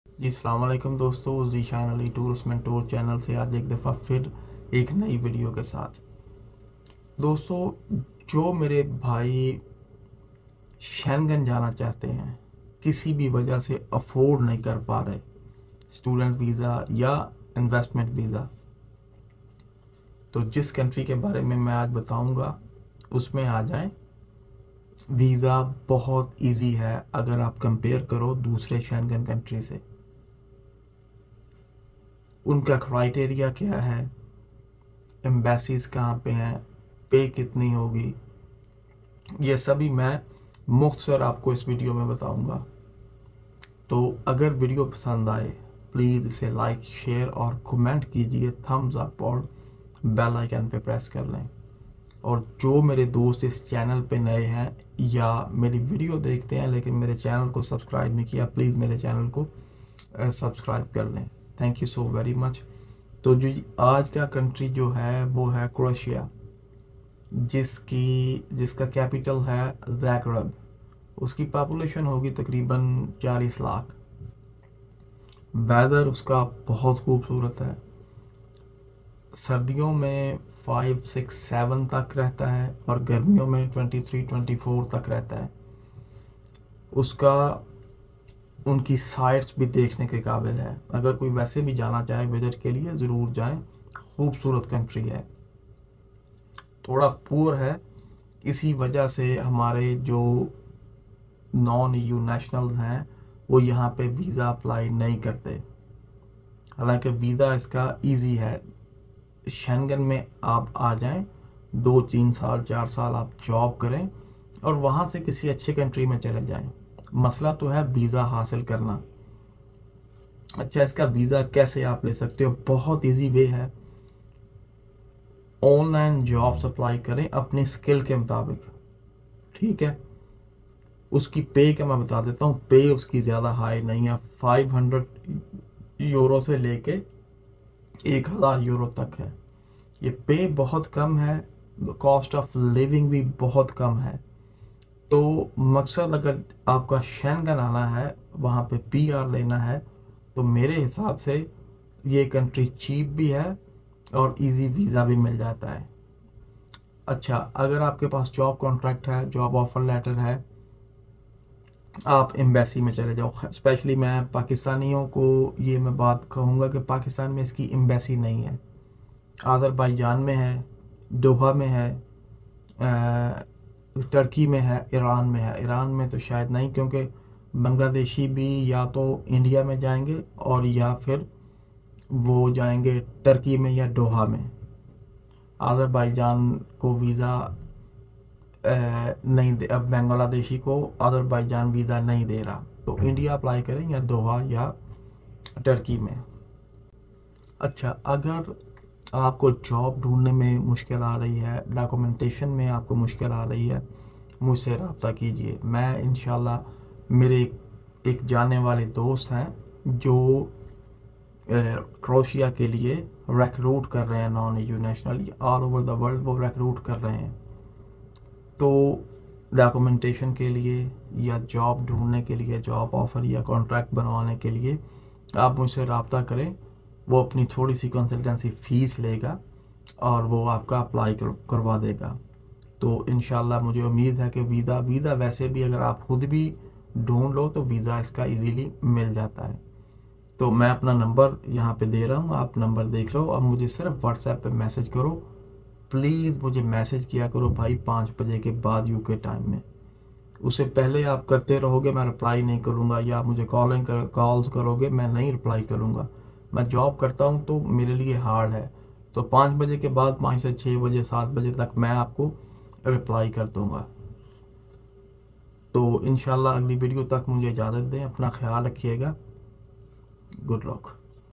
Croatia Visa Granted Easily Than Other Schengen States | 100% Acceptance Rate
Hello friends,
In this video i will guide you about a schengen state, country is very beautiful to live, visa acceptance ratio is almost 100%, video will be very informative, watch full video and share if you like it, thank you!

tourist mentor
croatia visa acceptance ratio
Best option to apply schengen visa
Browse features
Work permit
Study visa
Visit visa
Suggested
Tourism